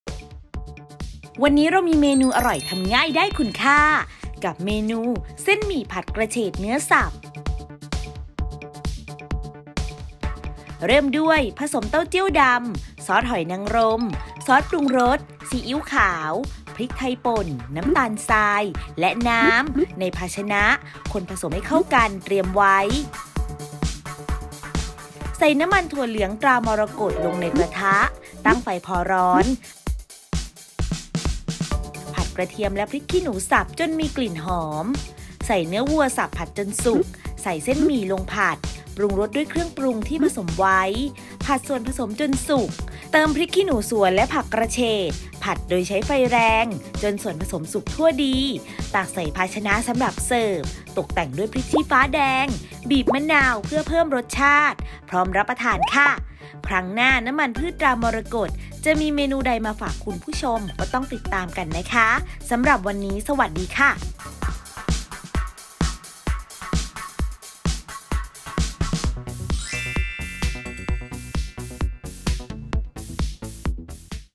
เส้นหมี่ผัดกระเฉดเนื้อสับ สูตรเด็ด ทำง่าย สไตล์ "อ.ยิ่งศักดิ์"
เส้นหมี่ผัดกระเฉดเนื้อสับ
Stir fried rice vermicelli with water mimosa and beef

ผลิตภัณฑ์ : น้ำมันถั่วเหลือง ตรามรกต

น้ำมันถั่วเหลือง ตรามรกต สำหรับผัด  3 ช้อนโต๊ะ
เต้าเจี้ยวดำ 1 ช้อนโต๊ะ
กระเทียมสับละเอียด  2 ช้อนโต๊ะ
พริกขี้หนูแดง สับละเอียด  2 ช้อนโต๊ะ
เนื้อวัวสับหยาบ  100 กรัม
พริกขี้หนูสวนบุบพอแตก  10 กรัม
เส้นหมี่ แช่น้ำจนนุ่ม  200 กรัม
ยอดอ่อนผักกระเฉด  150 กรัม
พริกชี้ฟ้าแดงหั่นเป็นเส้นสำหรับแต่งหน้า
มะนาวสำหรับปรุงรส

1.ผสมเต้าเจี้ยวดำ ซอสหอยนางรม ซอสปรุงรส ซีอิ๊วขาว พริกไทยป่น น้ำตาลทราย และน้ำ ในภาชนะคนผสมให้เข้ากันเตรียมไว้
2.ใส่น้ำมันถั่วเหลือง ตรามรกตลงในกระทะ ตั้งไฟพอร้อน ผัดกระเทียมและพริกขี้หนูสับจนมีกลิ่นหอม
3.ใส่เนื้อวัวสับผัดจนสุกใส่เส้นหมี่ลงผัด ปรุงรสด้วยเครื่องปรุงที่ผสมไว้ ผัดส่วนผสมจนสุก
4.เติมพริกขี้หนูสวนและผักกระเฉดผัดใช้ไฟแรง จนส่วนผสมสุกทั่วดี
ตักใส่ภาชนะสำหรับเสิร์ฟ ตกแต่งด้วยพริกชี้ฟ้าแดง บีบมะนาวเพื่อเพิ่มรสชาติพร้อมรับประทาน

▲Line @yingsakfood